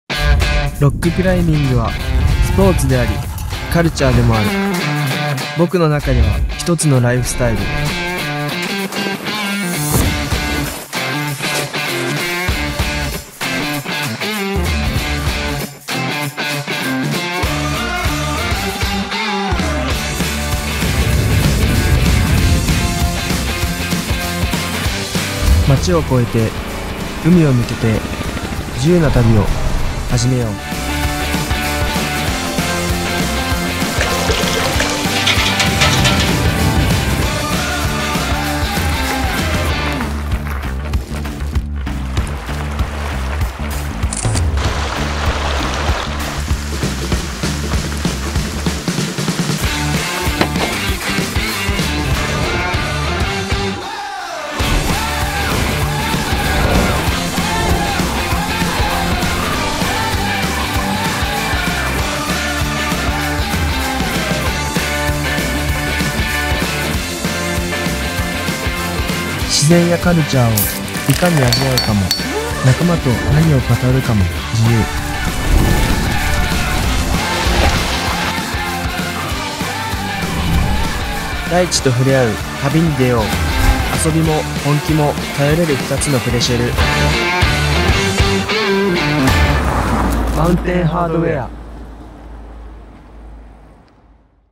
Mountain Hardware "Preshell" 2020 Japan ｜ Filmed with LUMIX S1H
Client : Columbia Sportswear Japan
Agency : ASAKO(Asahi Advertising Inc.)

CD/Writer, Director, DP, Editor : Osamu Hasegawa
Producer : Ray Kikuchi
Production manager / Assistant director : Daiki Mizoe
BTS camera, 2nd camera : T. Smith

Rock climber : Daisuke Ichimiya and Eiji Nomura

Filmed with
 - Panaosnic LUMIX S1H (1st cam)
 - Panaosnic LUMIX S1 (2nd cam)
 - Panaosnic LUMIX GH5 (Hand-held long shots)

Lenses : (with S1H/S1)
 - LEICA SUPER-VARIO-ELMAR-TL F3.5-4.5/11-23mm
 - Voigtländer NOKTON 40mm F1.2 VM
 - SIGMA 14-24mm F2.8 DG DN L-mount
 - LUMIX S PRO 70-200mm F4
 - LUMIX S PRO 24-70 mm F2.8

Lenses: (with GH5)
 - LEICA DG VARIO-ELMARIT 50-200mm / F2.8-4.0

Stabilizer
 - GLIDECAM HD4000
 - GLIDECAM Devin Graham Signature Model
 - DJI Ronin-S